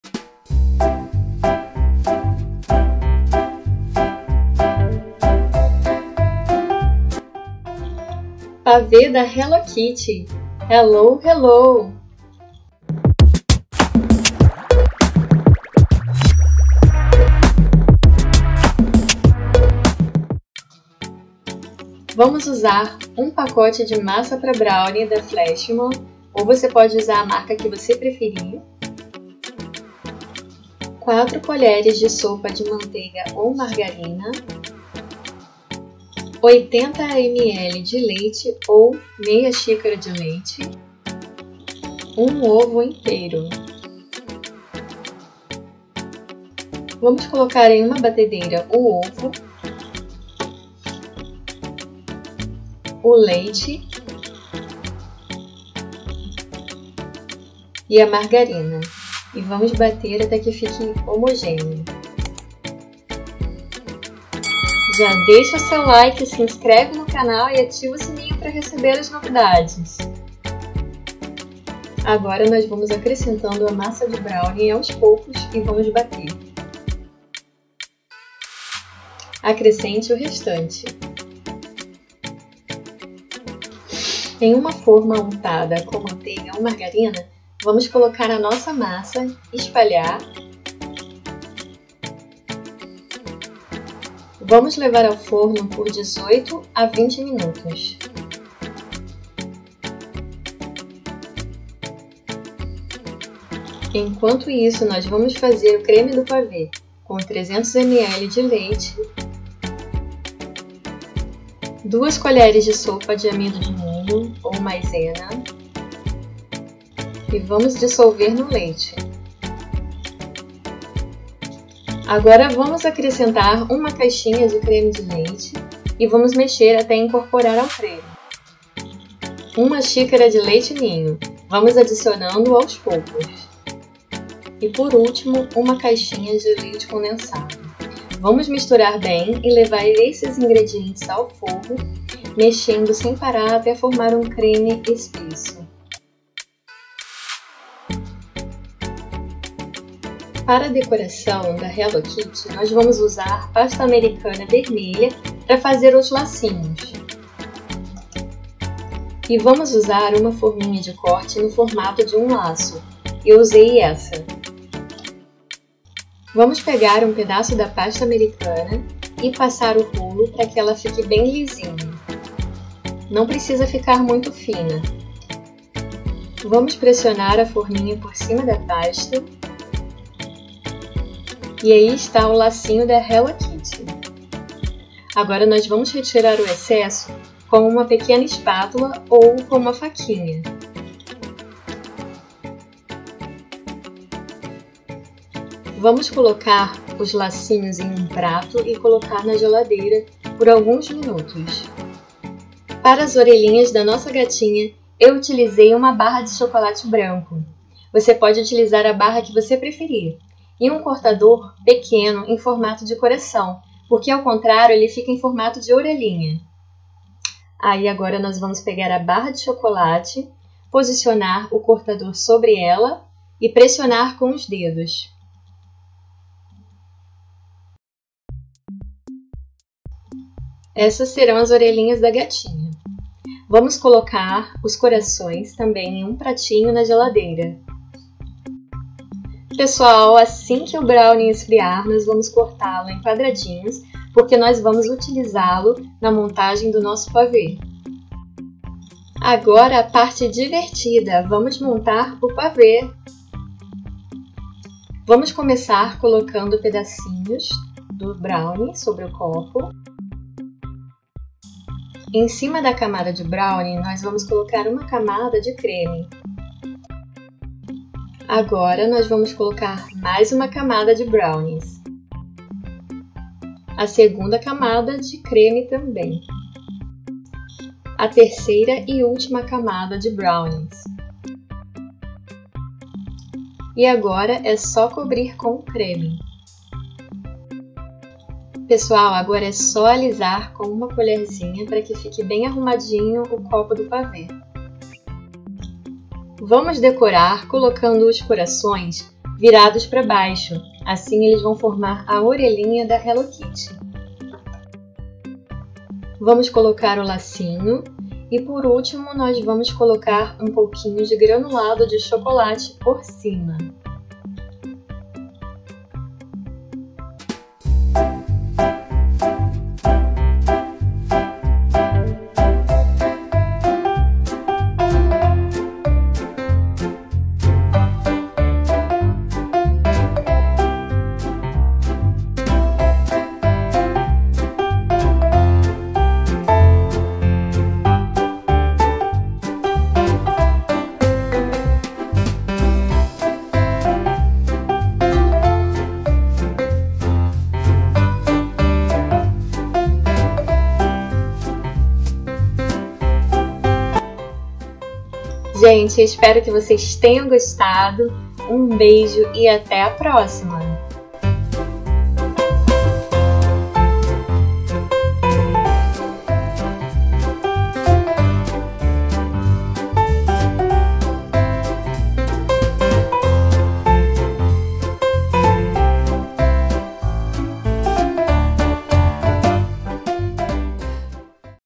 PAVÊ DE BROWNIE DA HELLO KITTY
❤ Oi pessoal, hoje trago o vídeo: PAVÊ DE BROWNIE DA HELLO KITTY

Receita de pavê de brownie da gatinha mais fofa dos desenhos, a Hello Kitty! Simples e fácil!

Ingredientes do Brownie:

- 1 ovo;
 - 1 pacote de massa para brownie;
- 80 ml de leite;
- 4 colheres de sopa de manteiga ou margarina.

Ingredientes do creme do pavê:

- 300 ml de leite;
- 2 colheres de sopa de amido de milho;
- 1 caixa de creme de leite;
- 1 xícara de leite Ninho;
- 1 caixa de leite condensado.

Decoração:

- Pasta americana vermelha;
- 1 barra de chocolate branco;
- Granulado de chocolate.

DEUS ABENÇOE CADA UM E CADA UMA DE VOCÊS ❤

➡ Não esqueça de comentar o que achou do vídeo